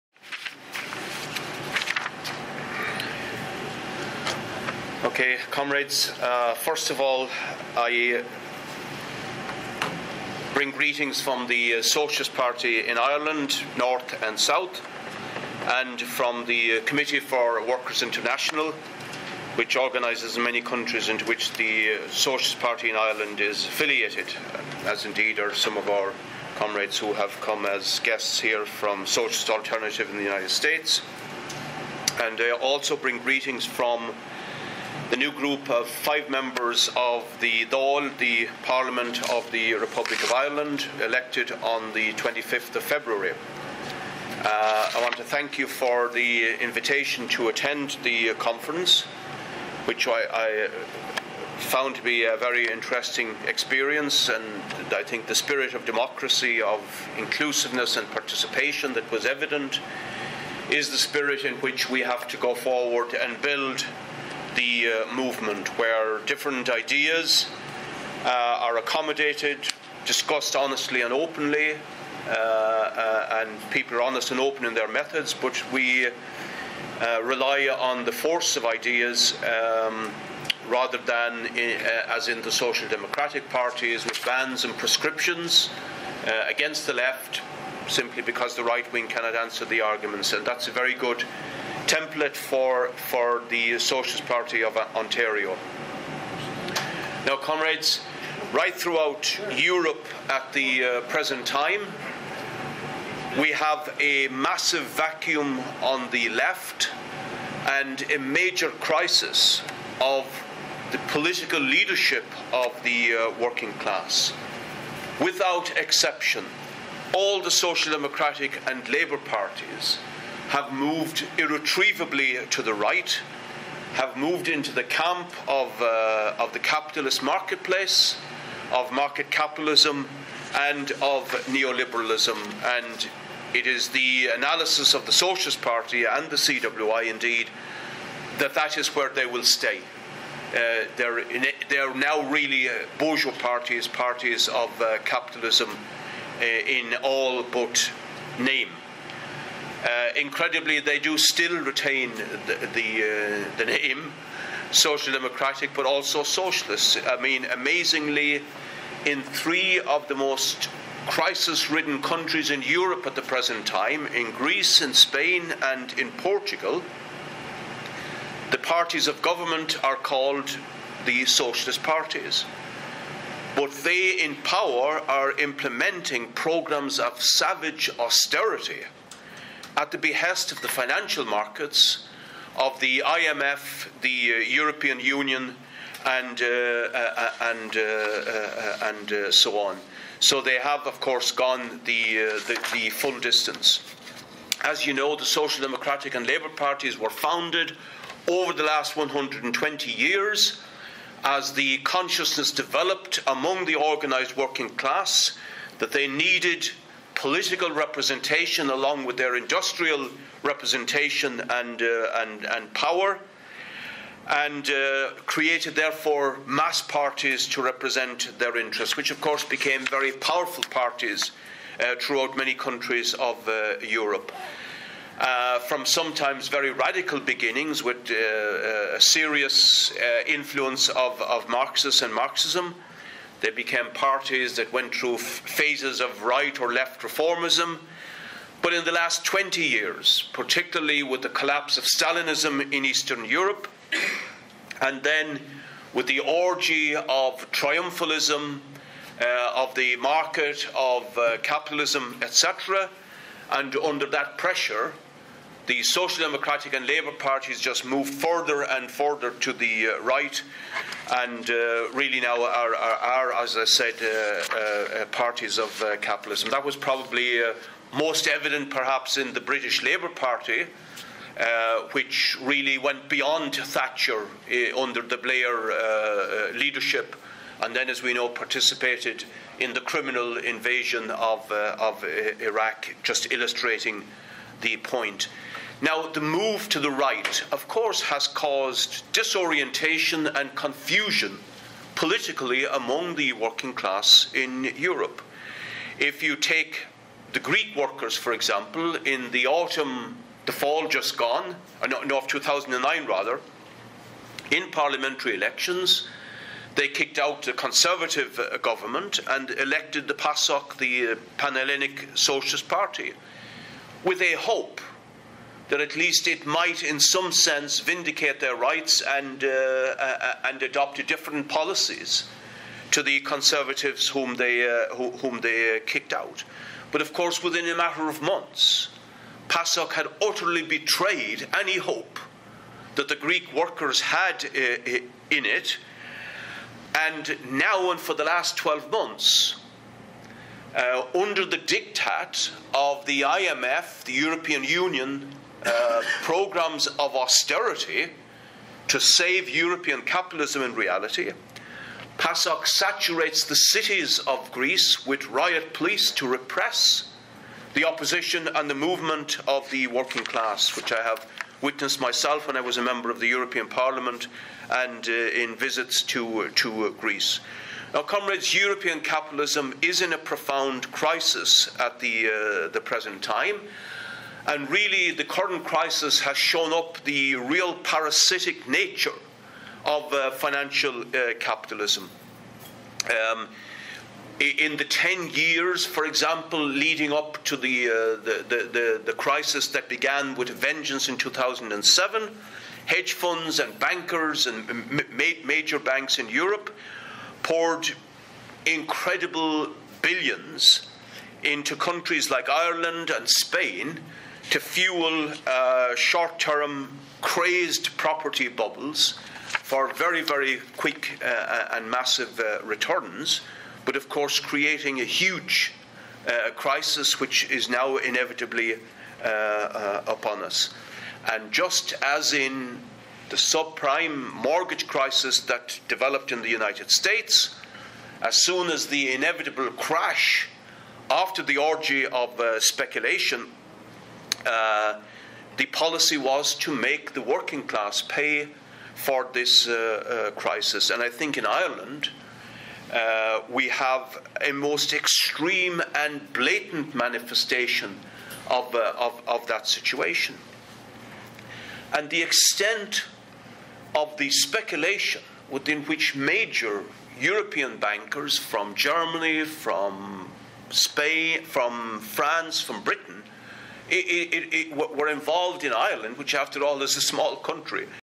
Joe Higgins Toronto Speech Part 1 of 4.mp4
Joe Higgins, a Socialist Member of Parliament in Ireland, speaking at the founding convention of the Socialist Party of Ontario.